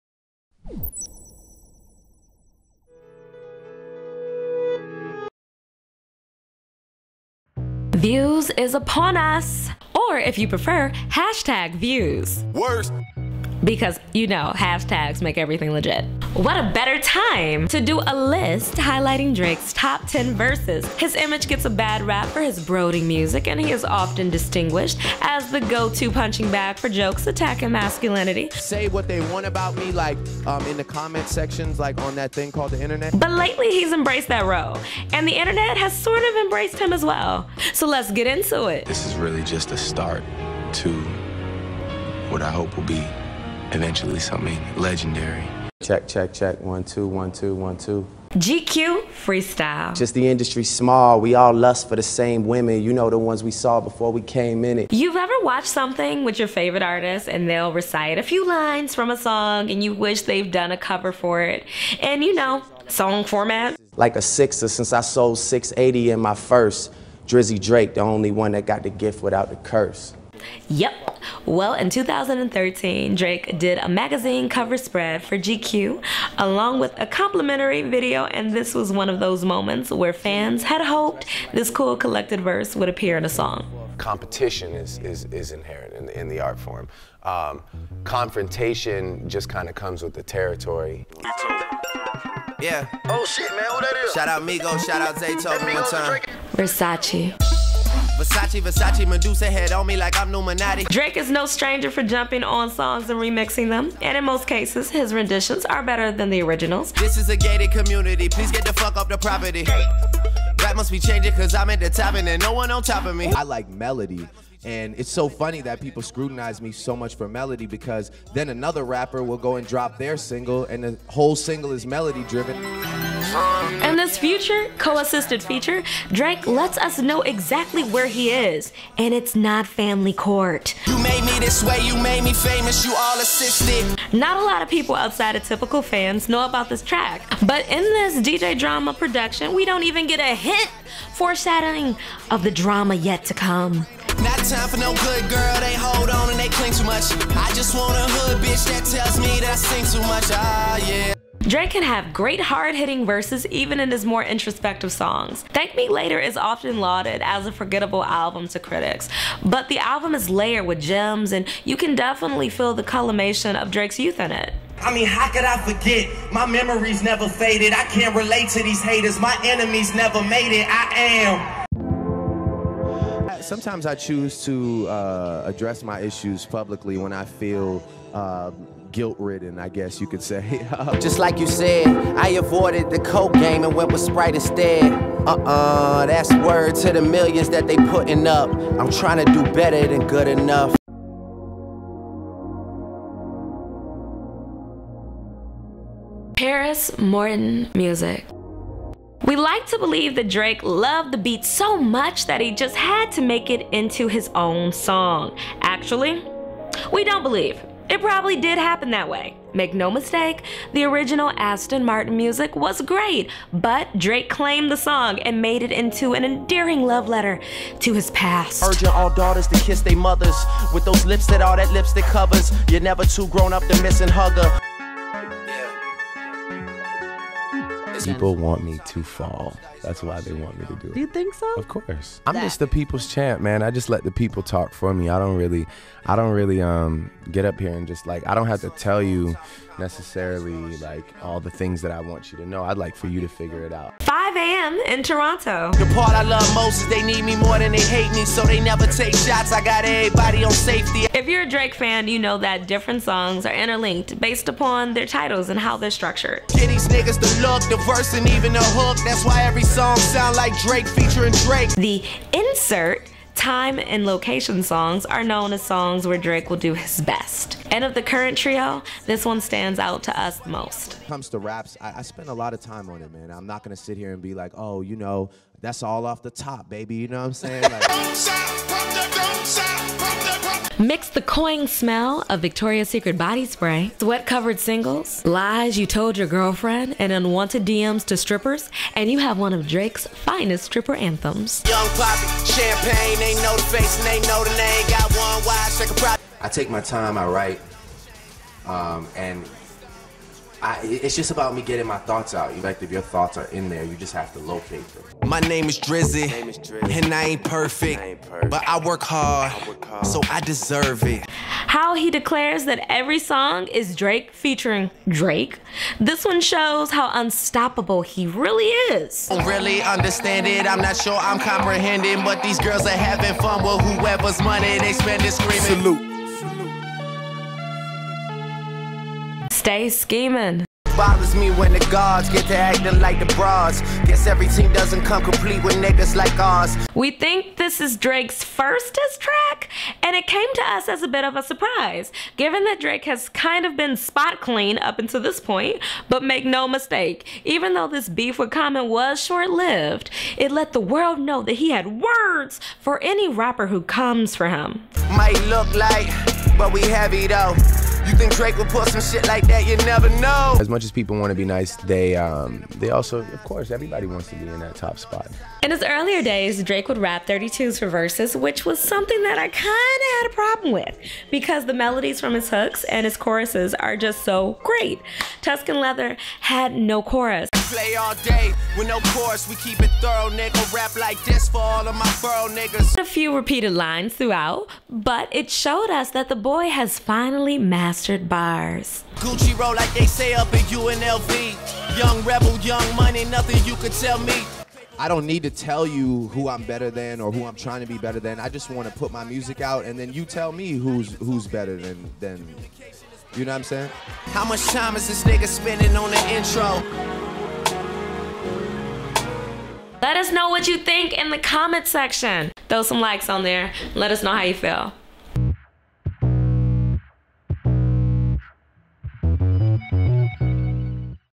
Dirty's Top 10 Drake Verses
What's your Top 10 Drake verses?